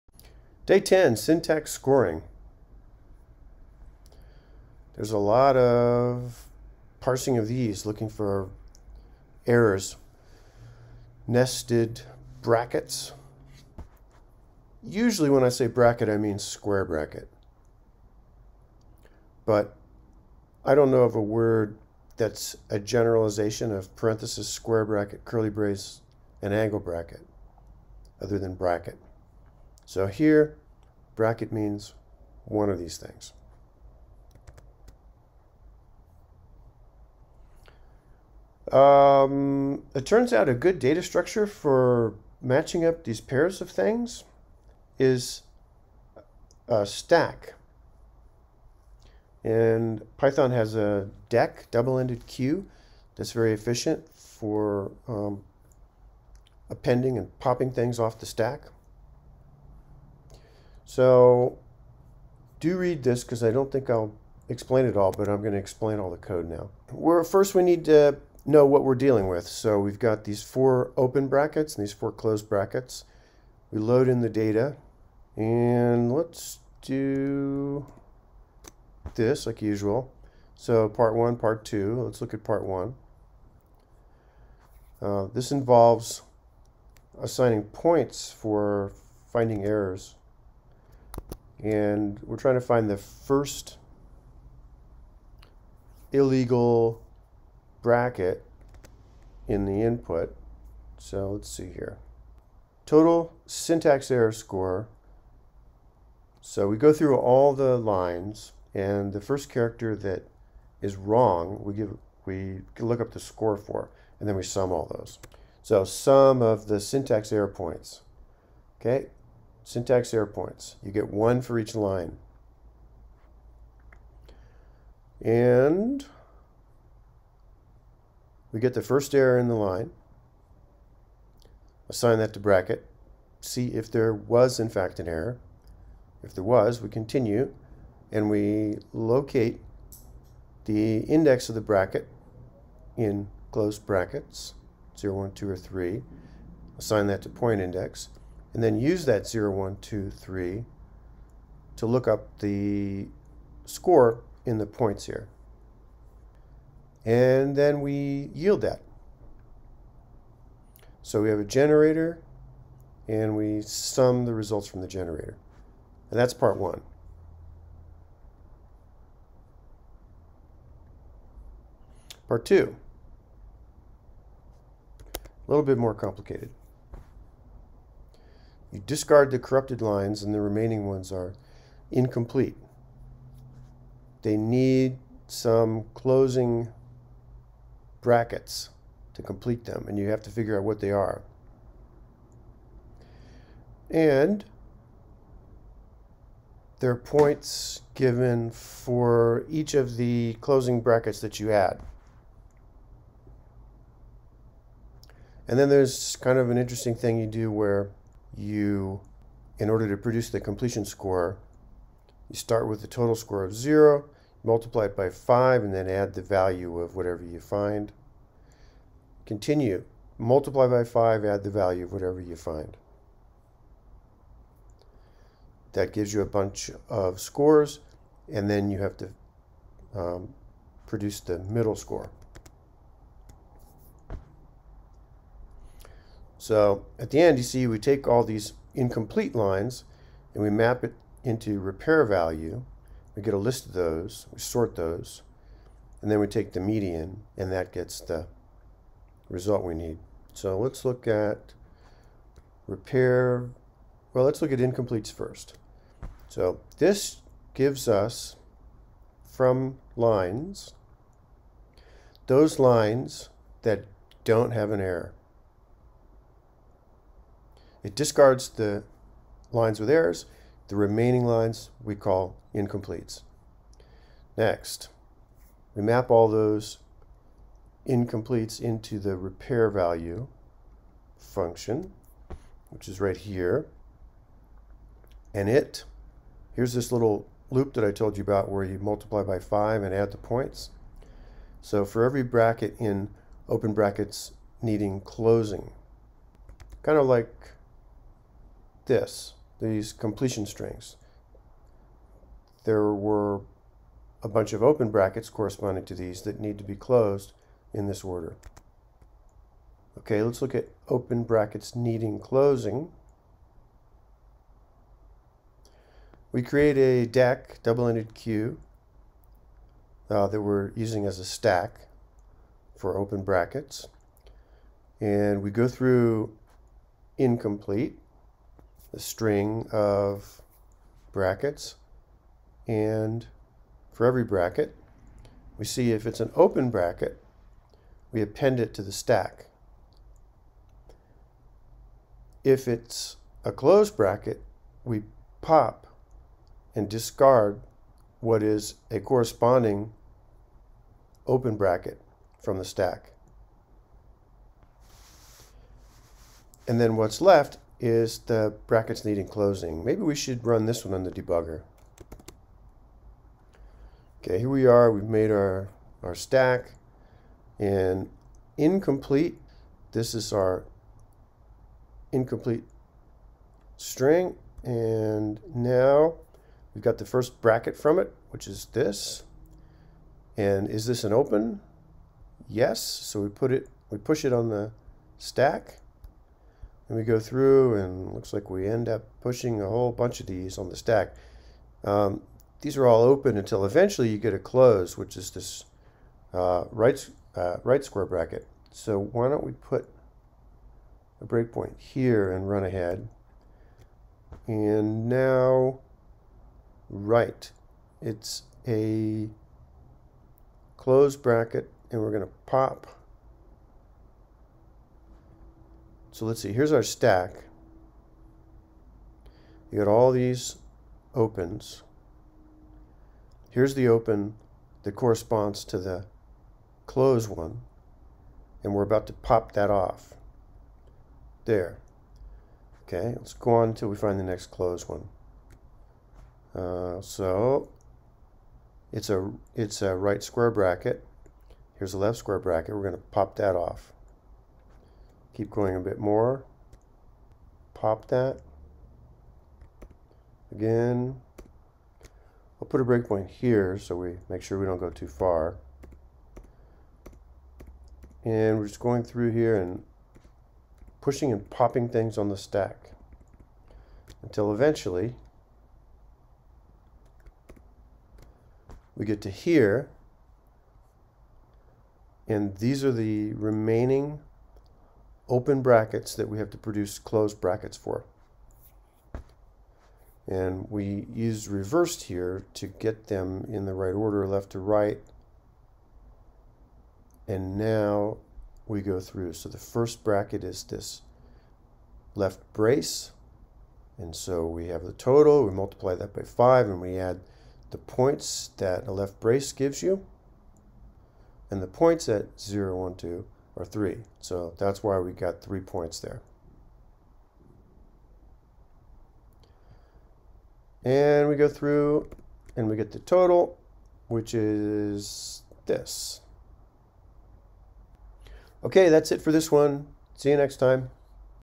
Advent of Code 2021 Day 10, Python Solution
Uses a deque (double-ended queue) as a stack for pushing and popping the open delimiters.